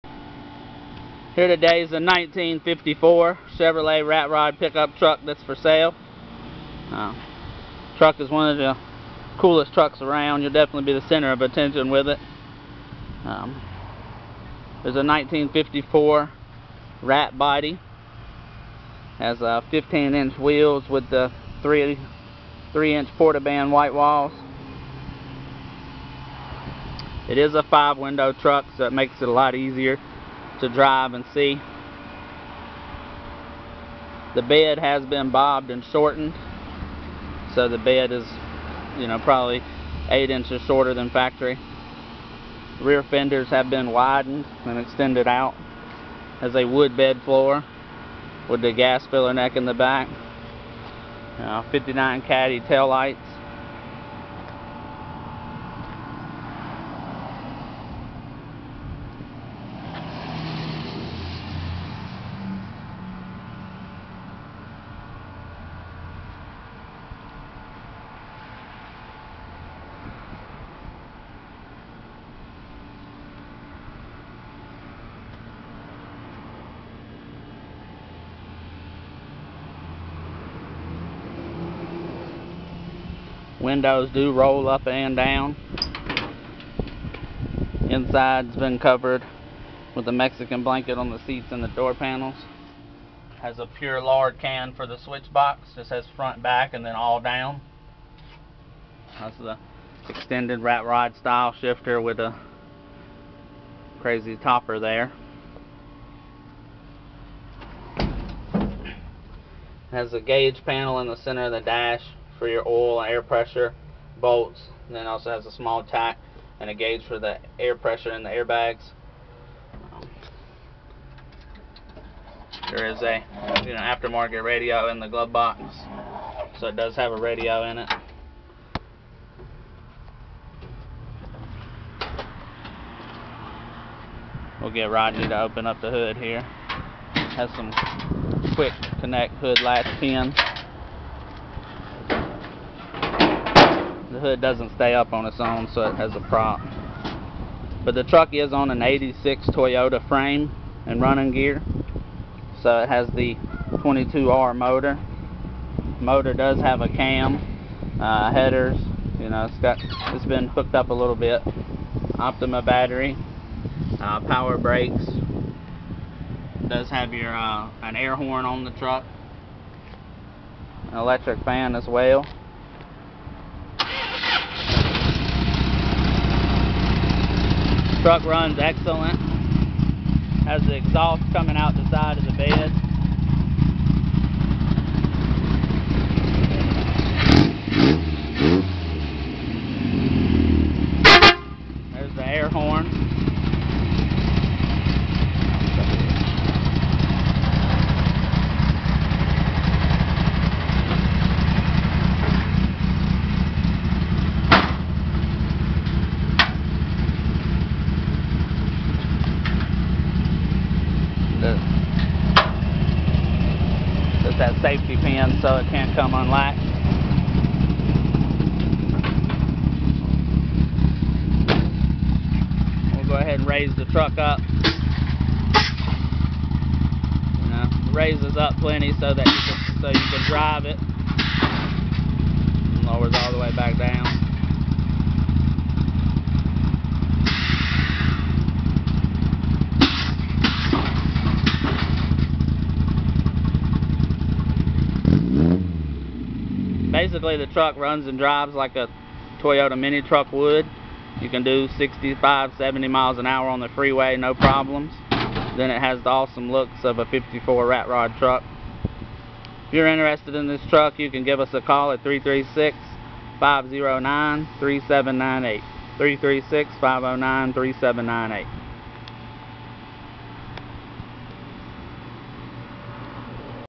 1954 Chevrolet Rat Rod pick up truck air bags Chevy bagged
this is a 1954 chevy rat rod truck. truck is way cooler then i can explain. the body sits on a 1986 toyota pick up truck frame that is fully bagged. body is body dropped and has have 3 inchs takin out to sit super low. the bed on the truck is bobbed for a shorter look. the truck uses all the toyota running gear so it has the great 22R motor with 4 speed trans. this means you have it all. looks of a bad ass rat rod chevy truck with the great running relieable motor of a toyota and saving on gas.